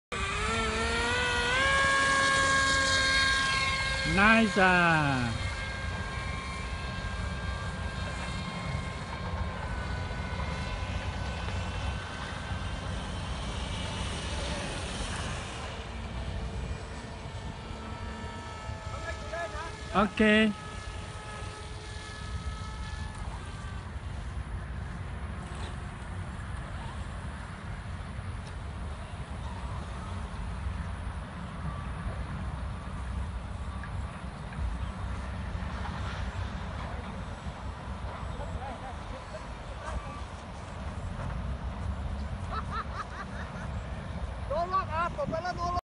2017年6月9日
Mr.Goh use truck to launch glider, well done!!!